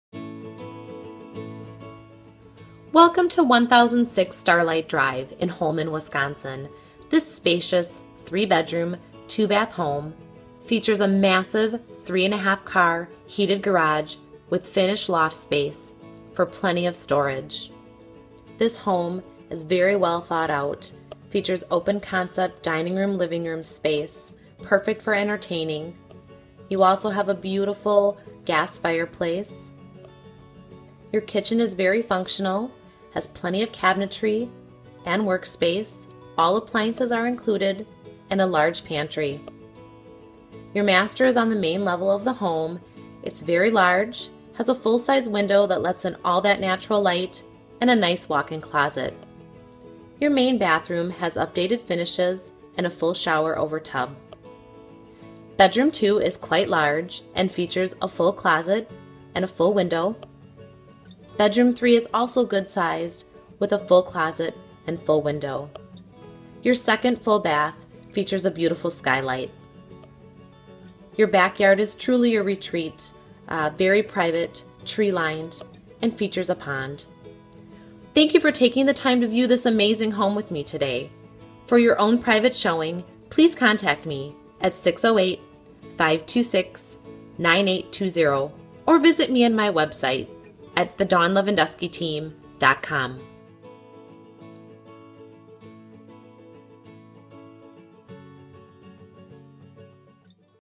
1006 Starlite Dt Holmen WI 54636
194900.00

SPACIOUS 3 BEDROOM 2 BATH HOME WITH MASSIVE 3.5 CAR HEATED GARAGE WITH FINISHED LOFT SPACE FOR PLENTY OF STORAGE FOR ALL YOUR TOYS. LARGE LIVING ROOM WITH GAS FIREPLACE. PRIVATE BACKYARD WITH POND & LARGE MATURE SHADE TREES THAT MAKES THIS THE PERFECT SPOT FOR ENTERTAINING. MAIN FLOOR MASTER WITH WIC. LARGE FUNCTIONAL MUDROOM. KITCHEN HAS PLENTY OF CABINETRY. SIX PANEL DOORS THROUGHOUT. FULL BASEMENT FOR FUTURE EXPANSION. MOVE-IN & ENJOY. CLOSE TO NEIGHBORHOOD PARKS, SCHOOL & SHOPPING

Let the Dawn Levandoski Team show you this great property today

To view all of our great properties